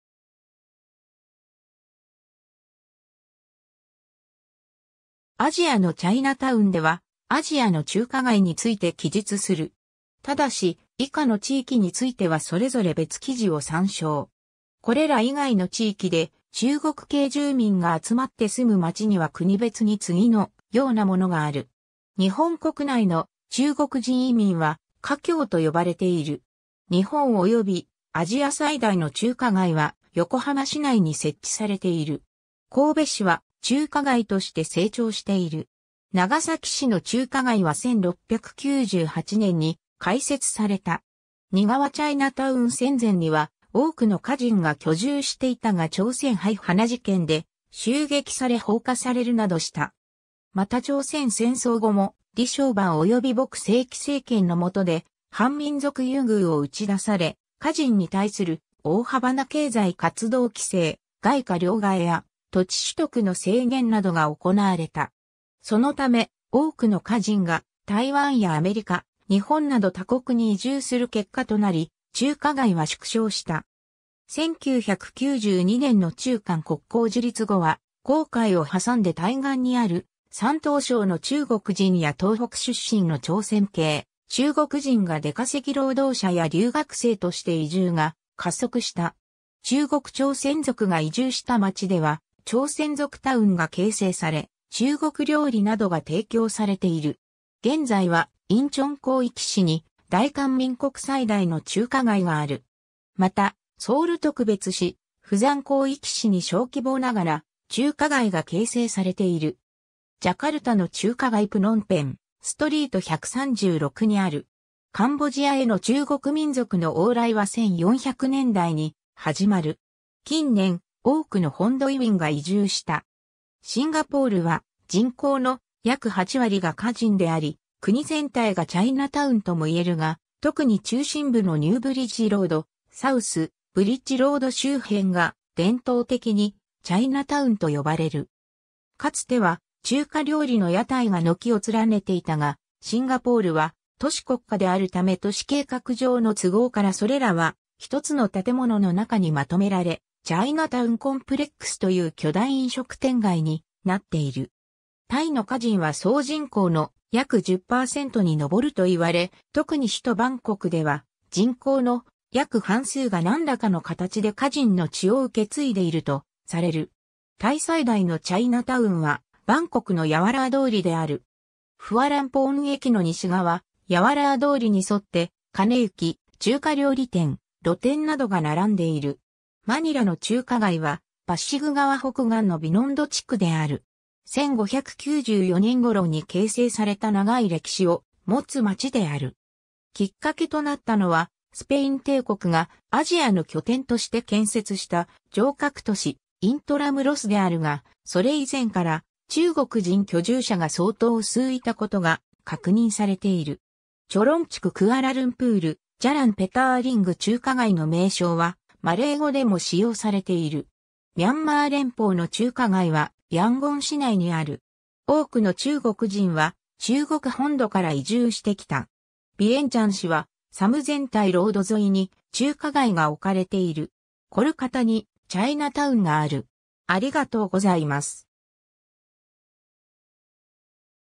アジアのチャイナタウン
アジアのチャイナタウン（Chinatowns in Asia）では、アジアの中華街（チャイナタウン）について記述する。
 ただし、以下の地域についてはそれぞれ別記事を参照。
 これら以外の地域で、中国系住民が集まって住む街には国別に次のようなものがある。
 日本国内の、中国人移民は華僑と呼ばれている。
 日本及びアジア最大の中華街は横浜市内に設置されている（参照：横浜中華街、浜町）。
神戸市は中華街として成長している（南京町）。
長崎市の中華街は1698年に開設された（長崎新地中華街）。
 仁川チャイナタウン 戦前には多くの華人が居住していたが朝鮮排華事件で襲撃され放火されるなどした。
また朝鮮戦争後も李承晩及び朴正煕政権の元で韓民族優遇を打ち出され、華人に対する大幅な経済活動規制、外貨両替や土地取得の制限などが行われた。
そのため、多くの華人が台湾やアメリカ、日本など他国に移住する結果となり、中華街は縮小した。
 1992年の中韓国交樹立後は、黄海を挟んで対岸にある山東省の中国人や東北出身の朝鮮系中国人（中国朝鮮族）が出稼ぎ労働者や留学生として移住が加速した。
中国朝鮮族が移住した街では「朝鮮族タウン」が形成され、中国料理などが提供されている。
 現在は仁川広域市に大韓民国最大の中華街（仁川チャイナタウン）がある。
 またソウル特別市、釜山広域市に小規模ながら中華街が形成されている。
 ジャカルタの中華街 プノンペン、Street 136 にある。
 カンボジアへの中国民族の往来は1400年代に始まる。
近年、多くの本土移民が移住した。
 シンガポールは人口の約8割が華人であり、国全体がチャイナタウンとも言えるが、特に中心部のニュー・ブリッジ・ロード、サウス・ブリッジ・ロード周辺が伝統的にチャイナタウンと呼ばれる。
かつては中華料理の屋台が軒を連ねていたが、シンガポールは都市国家であるため都市計画上の都合からそれらはひとつの建物のなかに纏められ、チャイナタウン・コンプレックスという巨大飲食店街になっている。
 タイの華人は総人口の約10%にのぼると言われ、特に首都バンコクでは人口の約半数が何らかの形で華人の血を受け継いでいるとされる。
タイ最大のチャイナタウンはバンコクのヤワラー通りである。
フワランポーン駅の西側、ヤワラー通りに沿って金行、中華料理店、露店などが並んでいる。
 マニラの中華街はパッシグ川北岸のビノンド地区である。
1594年頃に形成された長い歴史を持つ街である。
きっかけとなったのはスペイン帝国がアジアの拠点として建設した城郭都市｢イントラムロス｣であるが、それ以前から中国人居住者が相当数いた事が確認されている。
 チョロン地区(ホーチミン市) クアラルンプール、Jalan Petaling（地元広東語：chi cheong kai） 中華街（Chinatown）の名称はマレー語でも使用されている。
 ミャンマー連邦の中華街はヤンゴン市内にある。
多くの中国人は中国本土から移住してきた。
 ヴィエンチャン市はSamsenthai Road 沿いに中華街が置かれている。
 コルカタにチャイナタウンがある。